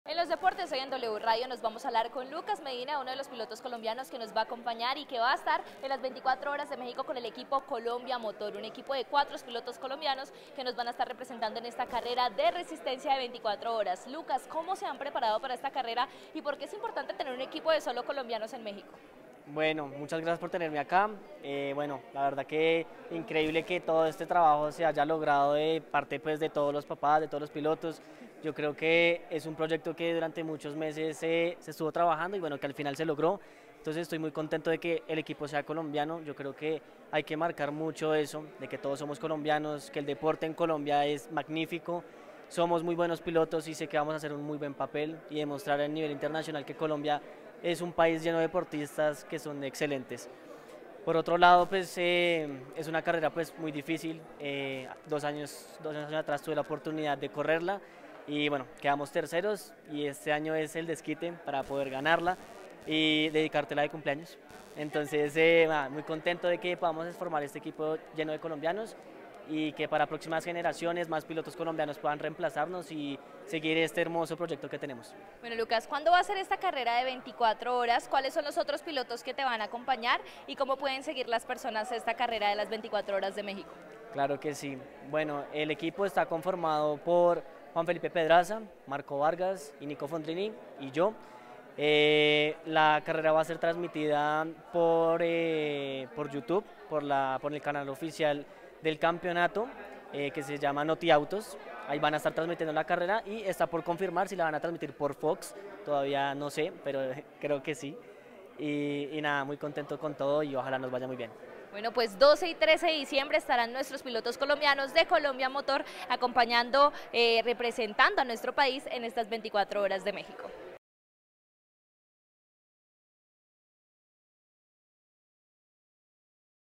El piloto colombiano Lucas Medina estara en las 24 horas de México
El piloto colombiano Lucas Medina estara en las 24 horas de México con el equipo Colombia Motor.